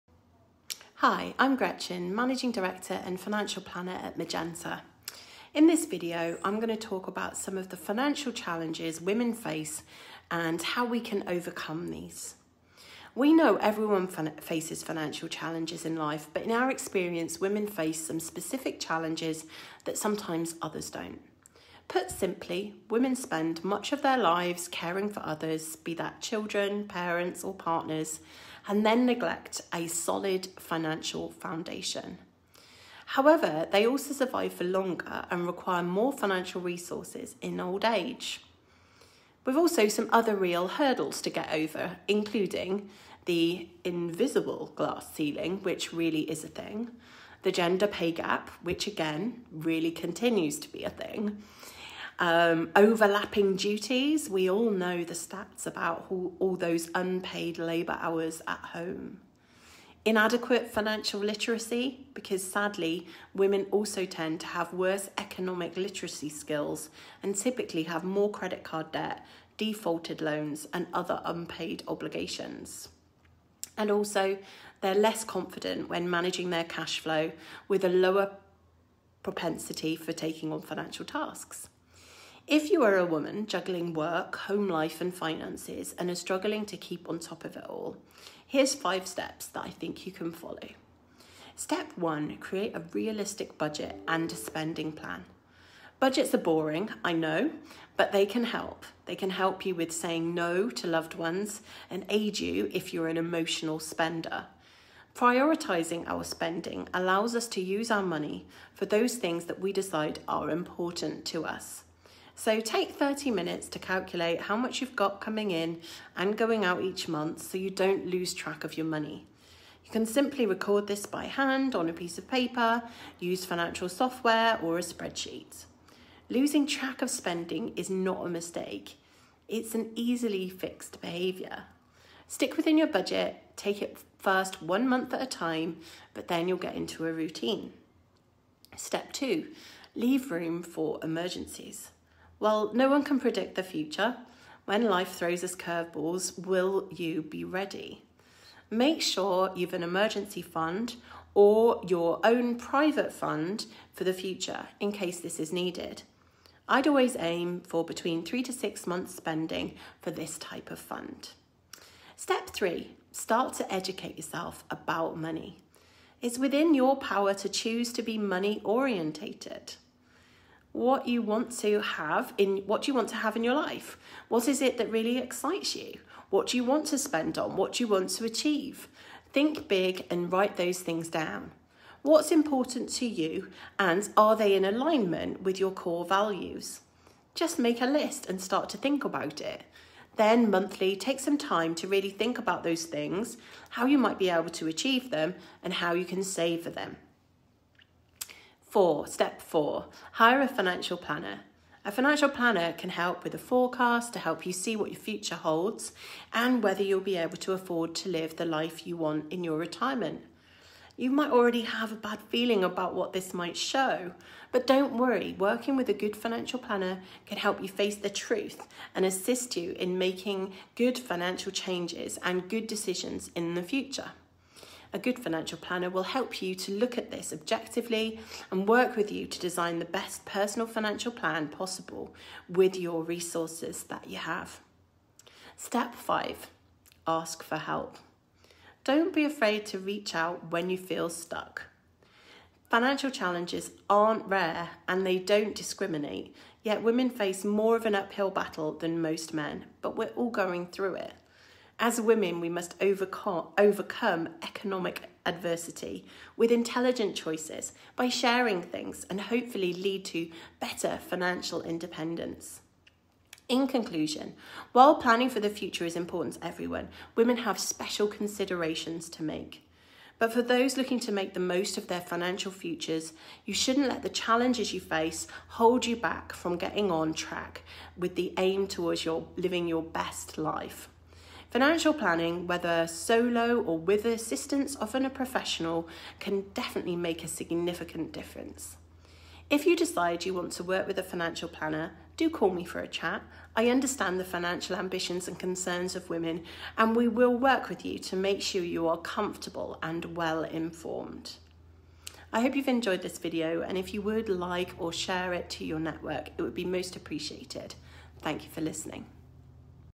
5 Common Financial Challenges Women Face
In this video we discuss 5 Common Financial Challenges Women Face.
Simply, women spend much of their lives caring for others, be that children, parents, or partners.

Subsequently they neglect creating a solid financial foundation. On top of that, women survive longer and require more financial resources in old age.

If you would like more information on this subject, or if we can help to support you, or someone that you love, please do get in touch.

If you have found this video of interest, please do like it and share it with your network 😊